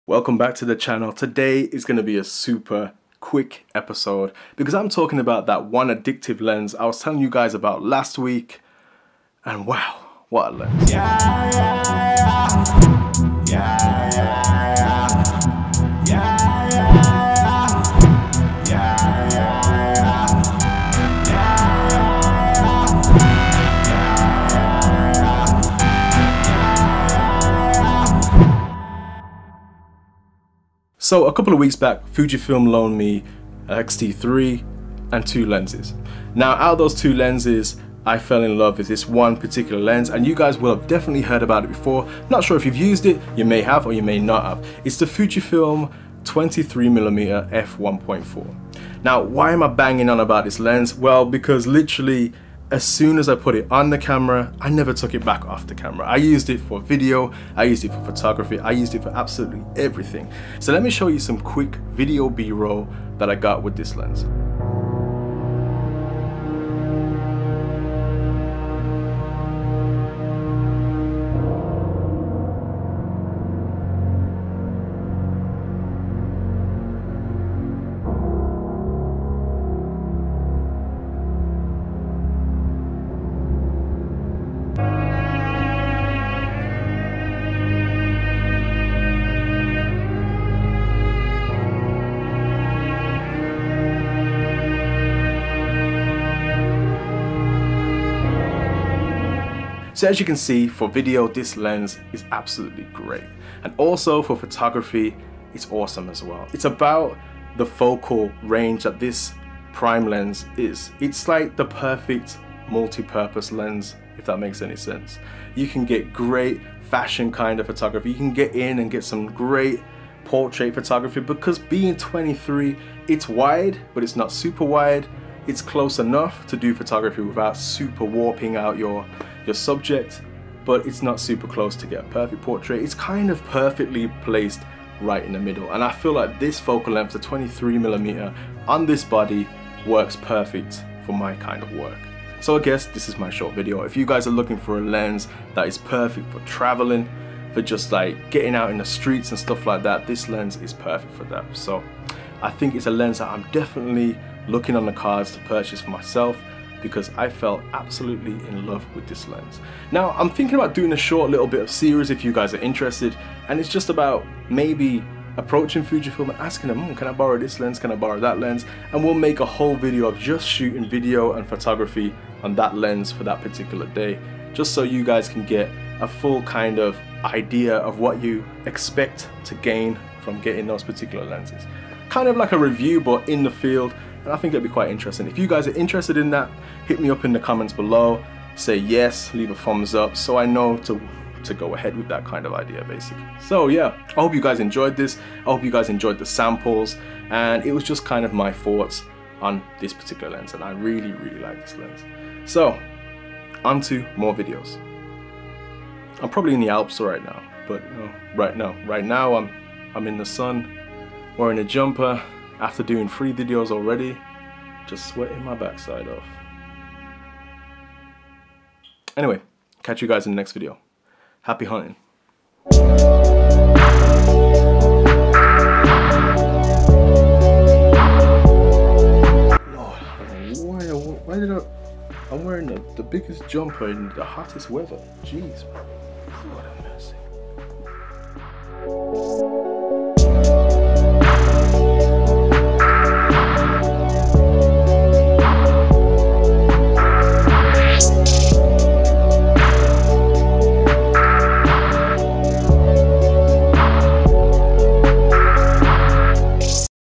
Fujifilm Has One Addictive lens!!
Fujifilm Has One Addictive lens!! the 23mm f1.4 wow i fell in love with this lens it had me from the off!! i mainly used it for video though i dont know why but it felt so natural shootings videos with it!!